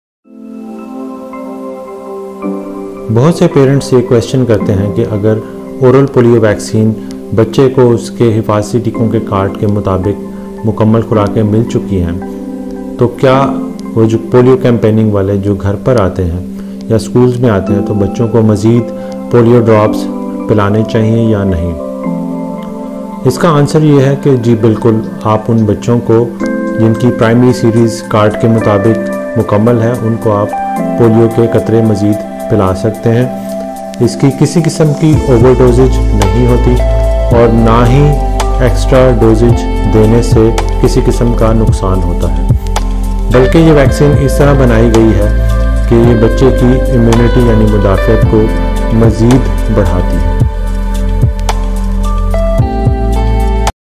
Is there any harm in giving extra doses of polio drops during campaining.. PART-1 POLIO DROPS MYTHS
Door to door polio campaigning and role of parents..
Should we go for extra polio drops during these campaigns